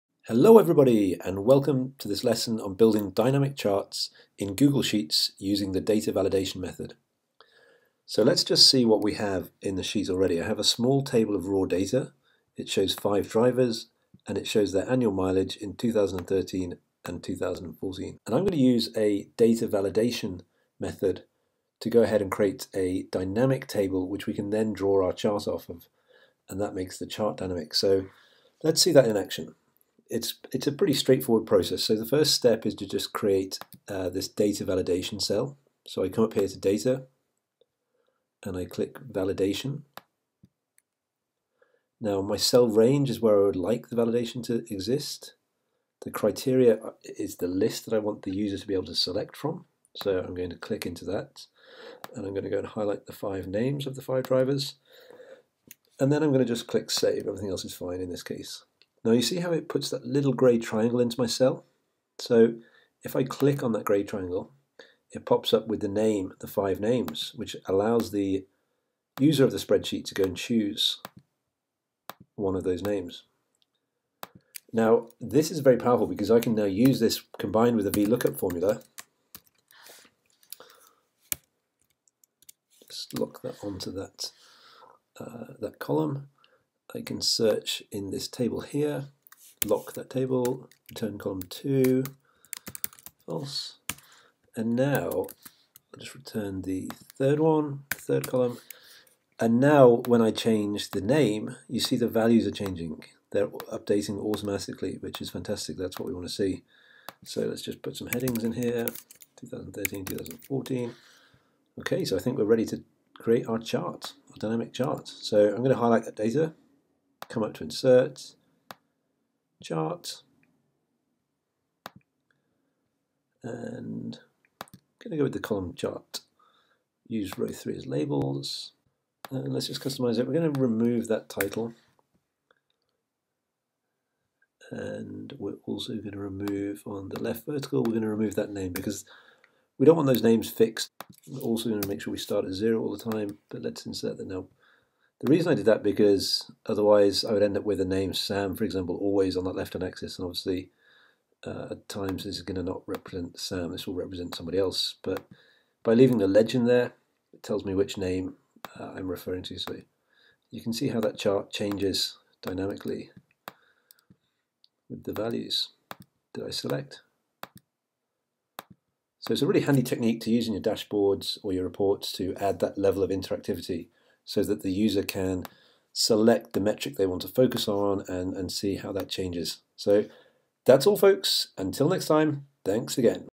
Dynamic charts in Google Sheets with data validation
A tutorial on how to create dynamic charts in Google Sheets using the data validation method and vlookup formulas.